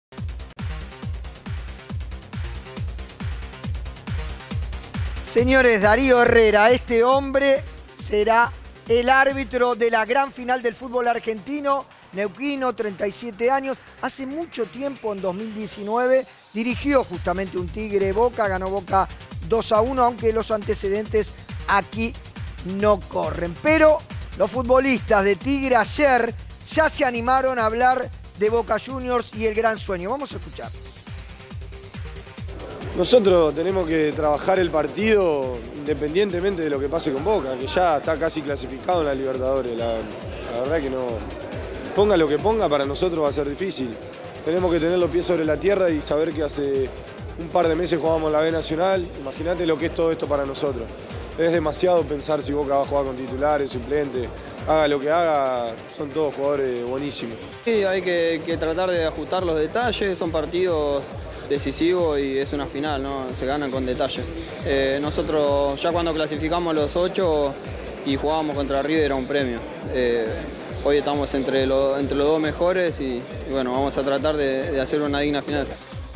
Darío Herrera, el árbitro de la gran final
La final de la Copa de la Liga será dirigida por Darío Herrera, uno de los árbitros de mejor rendimiento en los últimos tiempos junto a Patricio Loustau. Boca y Tigre definirán el torneo el domingo en Córdoba.